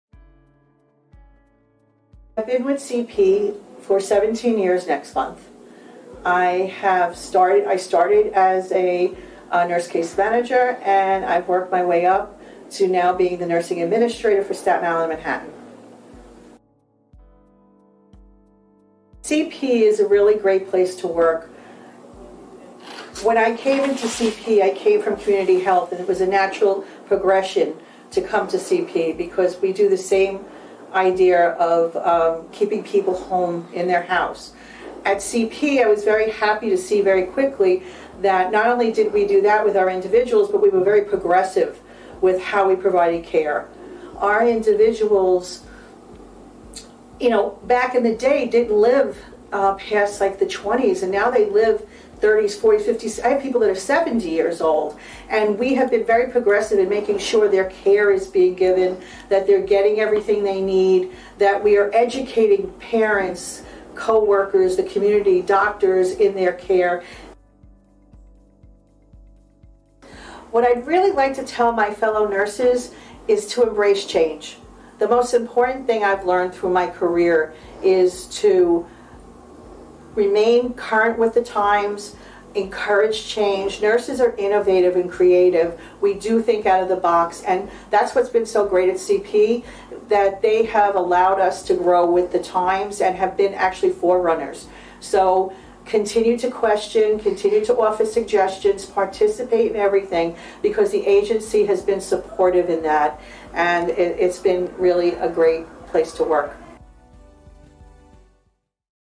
Celebrating CP Nurses: Joann DeGeorge
Joann DeGeorge, Nursing Administrator for Staten Island, with CP Unlimited for seventeen years.

In celebration and recognition of National Nurses Week 2023, we asked a selection of our nursing team to discuss nursing as a profession and here at CP Unlimited. Here is what they said.

Thank you to all of our wonderful nurses, whose endless passion is only matched by their deep knowledge and care for the people we support.